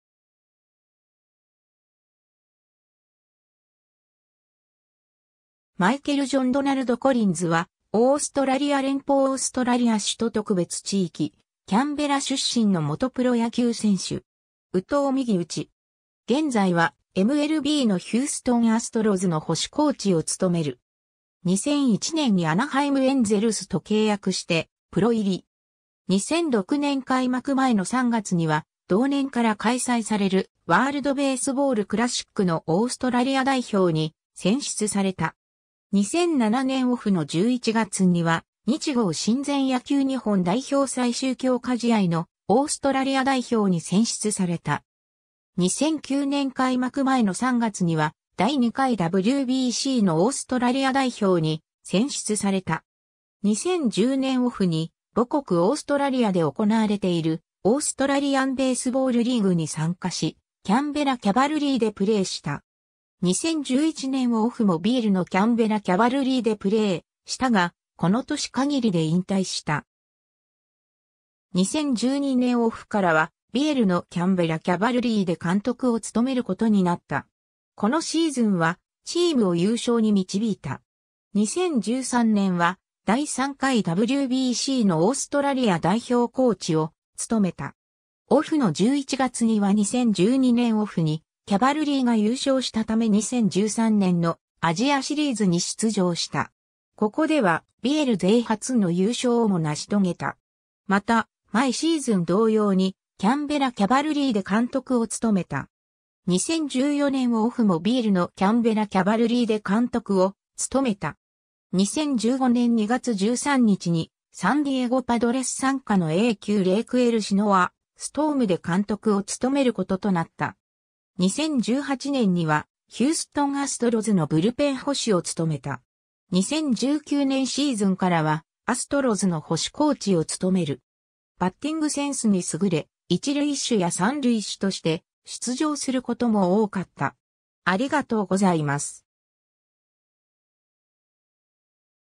マイケル・コリンズ (野球)
ヒューストン・アストロズ関連人物
ワールド・ベースボール・クラシック・オーストラリア代表選手
マイケル・ジョン・ドナルド・コリンズ（Michael John Donald Collins, 1984年7月18日 - ）はオーストラリア連邦オーストラリア首都特別地域キャンベラ出身の元プロ野球選手（捕手）。
右投右打。
現在はMLBのヒューストン・アストロズの捕手コーチを務める。
 2001年にアナハイム・エンゼルス（現：ロサンゼルス・エンゼルス）と契約してプロ入り。
 2006年開幕前の3月には同年から開催されるワールド・ベースボール・クラシック(WBC)のオーストラリア代表に選出された。
 2007年オフの11月には「日豪親善 野球日本代表最終強化試合」のオーストラリア代表に選出された。
 2009年開幕前の3月には第2回WBCのオーストラリア代表に選出された。
 2010年オフに母国オーストラリアで行われているオーストラリアン・ベースボールリーグ(ABL)に参加しキャンベラ・キャバルリーでプレーした。
 2011年オフもABLのキャンベラ・キャバルリーでプレーしたが、この年限りで引退した。
 2012年オフからはABLのキャンベラ・キャバルリーで監督を務める事になった。
このシーズンは、チームを優勝に導いた。
 2013年は第3回WBCのオーストラリア代表コーチを務めた。
オフの11月には2012年オフにキャバルリーが優勝したため2013年のアジアシリーズに出場した。
ここでは、ABL勢初の優勝をも成し遂げた。
また、前シーズン同様にキャンベラ・キャバルリーで監督を務めた。
 2014年オフもABLのキャンベラ・キャバルリーで監督を務めた。
 2015年2月13日にサンディエゴ・パドレス傘下のA+級レイクエルシノア・ストームで監督を務める事となった。
 2018年にはヒューストン・アストロズのブルペン捕手を務めた。
2019年シーズンからはアストロズの捕手コーチを務める。
 バッティングセンスに優れ、一塁手や三塁手として出場することも多かった。